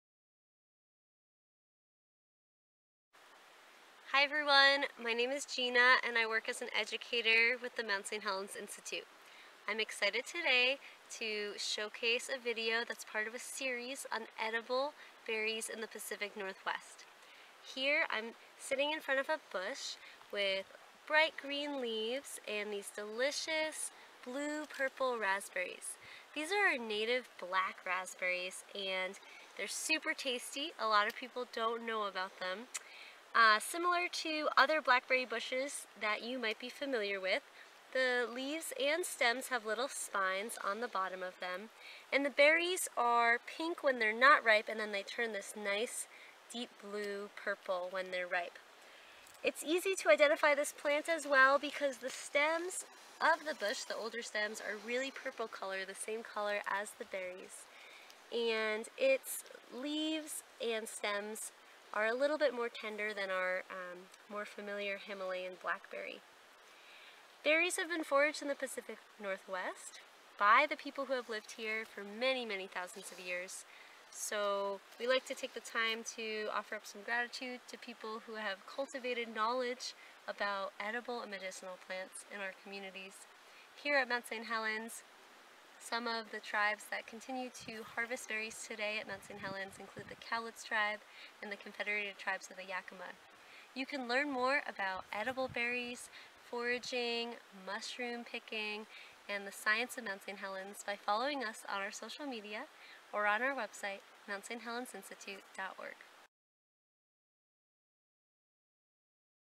Edible Berries of the Pacific Northwest | Black Raspberries
It's summertime in the Pacific Northwest and that means berry season! Learn about the diversity of native and delicious edible plants with these education shorts produced by staff at the Mount St. Helens Institute.

Black Raspberry (Rubus leucodermis) is also know as Whitebark Raspberry or Blackcap Raspbrerry is one of over 200 native species of berries native to North America in the Rubus genus which includes blackberries, thimbleberries, salmonberries and raspberries. The Black Raspberry is a native species that is found along the western coast of North America from southern California to southeastern Alaska.

Identification: This berry grows tall as a shrub with long whitish/purple stems that can grow as high as 6 feet tall. The scientific name for this species, Leucodermis, means "white skin" referring to the color of the stems. Leaves come off of the main stem clusters of three leaflets and have finely serrated margins. The underside of the leaves and stems have small spines. Berries turn dark purple when ripe.

Follow the Mount St. Helens Institute on social media or visit our to learn more about the science and stories of Mount St. Helens!